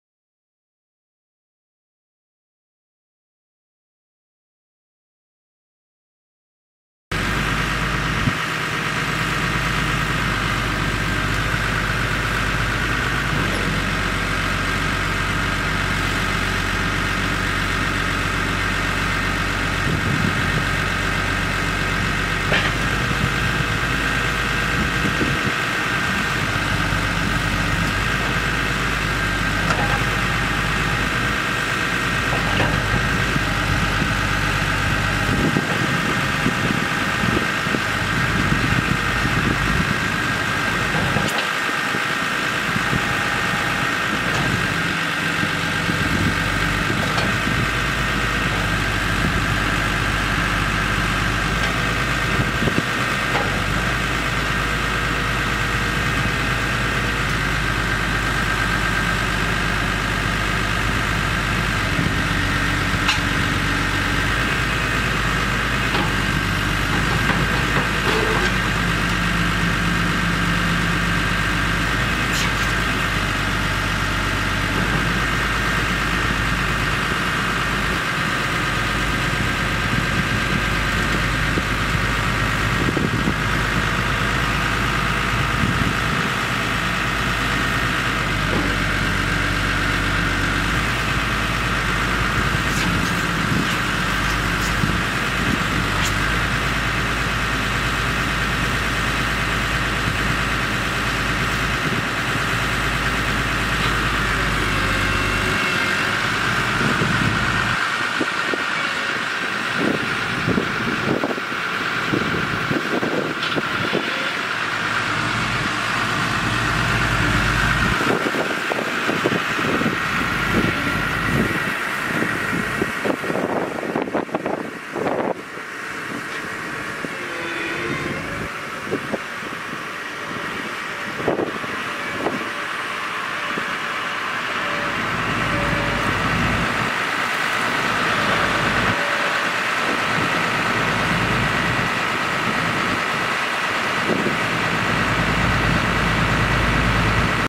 Case IH Maxxum 140 Limited series MFWD tractor for sale | no-reserve auction August 23, 2017
Case IH Maxxum 140 Limited series MFWD tractor
1,804 hours on meter
Six cylinder turbo diesel engine
Autoshift transmission
16F-16R gears
AC and heat
AM/FM
Suspension seat
M&W 1660 loader
96"W bucket
Quick attach
Grapple attachment
Joystick controls
Three point
1000 PTO
(3) hydraulic remote sets
380/85R39 front tires
460/85R42 rear tires
Item will be utilized for loadout and will be unavailable for removal until August 28.
It's located in Capron, OK, and we're selling it to the highest bidder regardless of price.